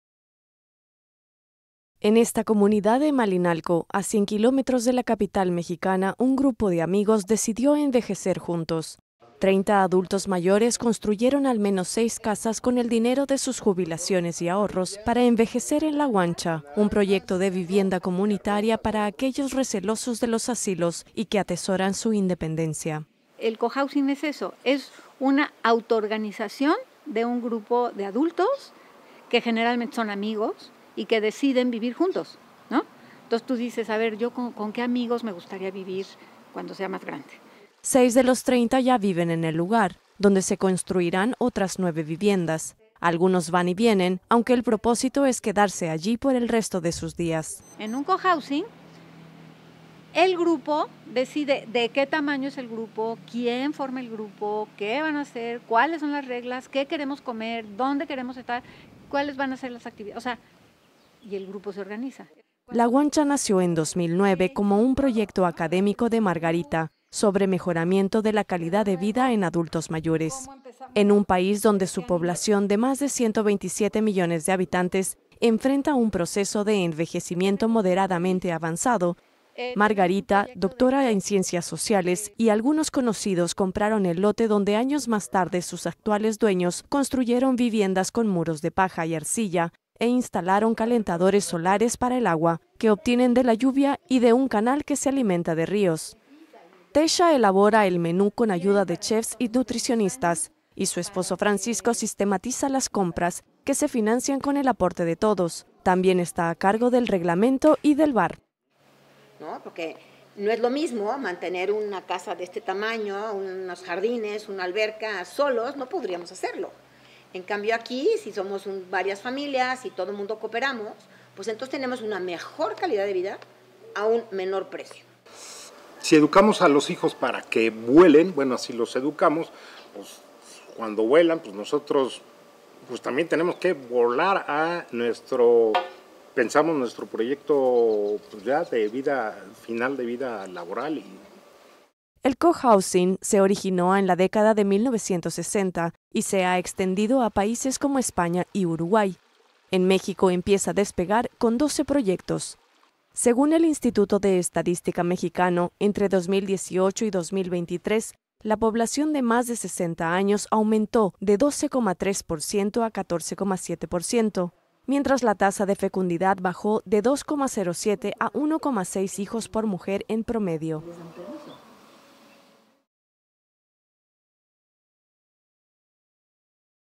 Cada vez más adultos mayores eligen envejecer en comunidad en México
En Malinalco, una ciudad a 100 kilómetros de la capital mexicana, un grupo de 30 amigos decidió envejecer en comunidad. Para conseguirlo, los adultos mayores construyeron seis casas con el dinero de sus ahorros y jubilaciones para vivir en “La Guancha”, nombre que le pusieron a su proyecto de vivienda comunitaria.